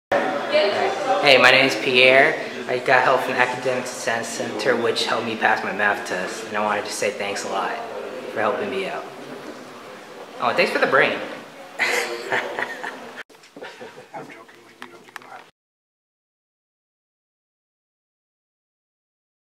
BCSouthCampus Academic Success Story - Pierre
We would like to thank Pierre, a student on ‪#‎BCSouth‬ who took the time to come by the ‪#‎ASC‬ and the Math Lab to thank everyone who helped him pass his math test.
Thank You Pierre
‪#‎BCProud‬
‪#‎BCMath‬

diverse, professional, student friendly, Pembroke Pines, FL